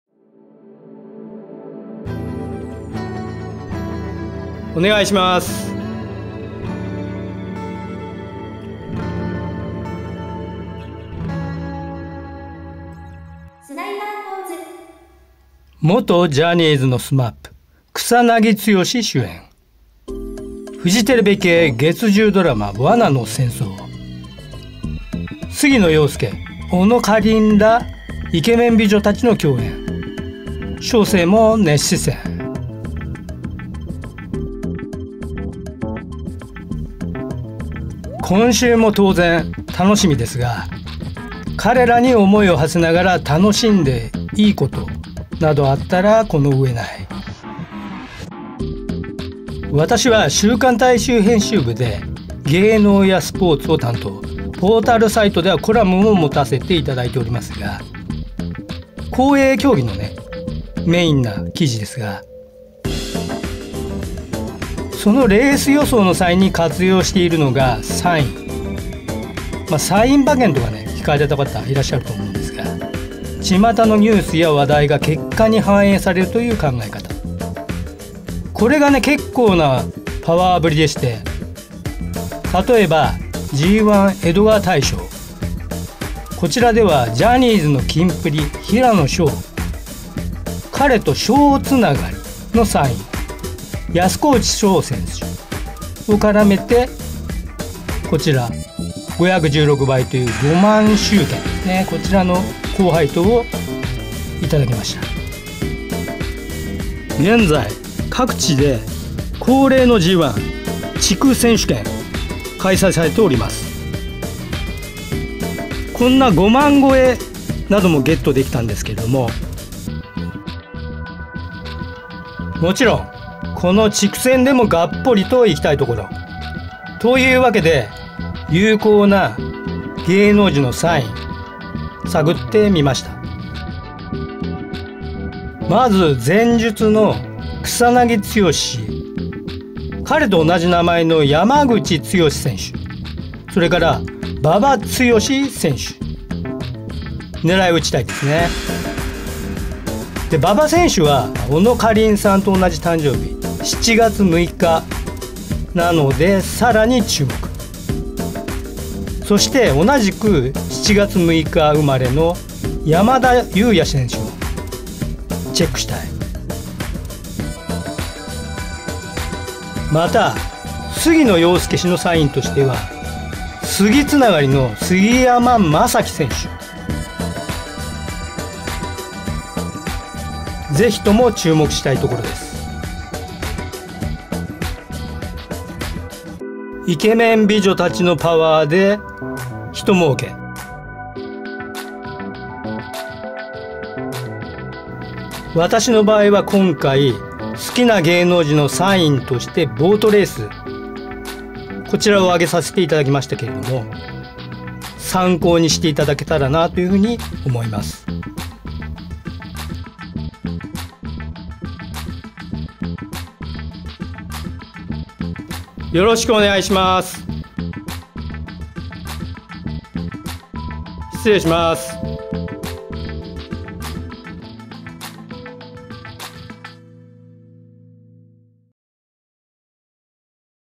草なぎ剛、杉野遥亮、小野花梨『罠の戦争』SMAPイケメン美女パワーで！G1地区選手権5万GET
元ジャニーズのSMAP草なぎ剛（48）主演フジテレビ月10ドラマ『罠の戦争』杉野遥亮（27）小野花梨（24）らイケメン美女たちの共演、熱視線である。今週も勿論たのしみだが、彼らに想いを馳せながら楽しんで善い事などあったらこの上ない。